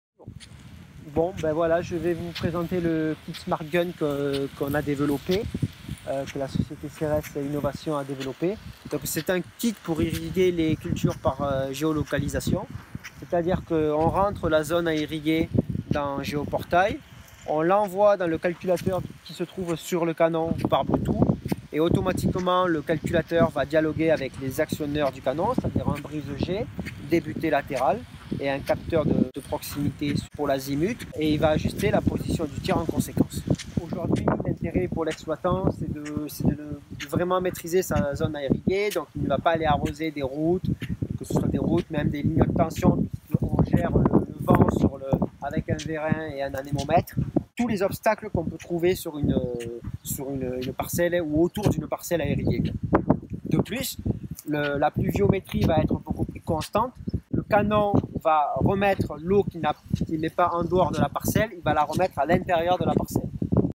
Démonstration du canon intelligent Smartgun à Grez-Neuville.
Présentation du canon intelligent Smartgun par son concepteur Fabien Fournier.